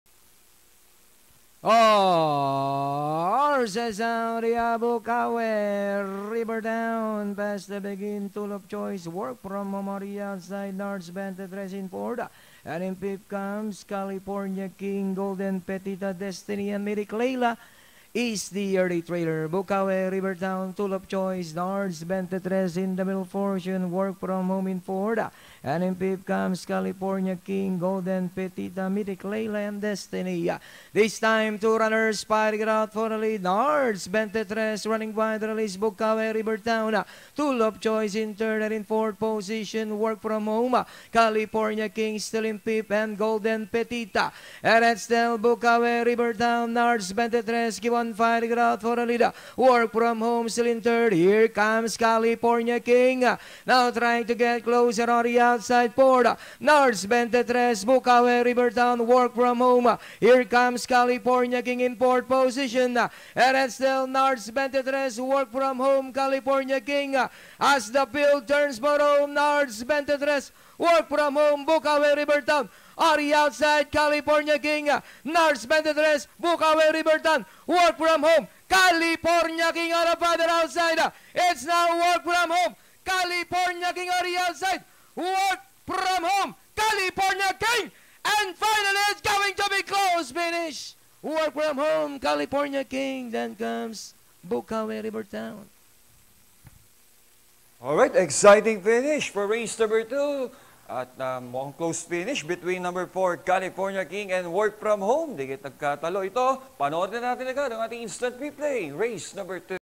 2024 July 13 | R02 - WORK FROM HOME (8) [1:28.2]
Winning Horse - WORK FROM HOME (8) [1:28.2]
Jockey : Y L BAUTISTA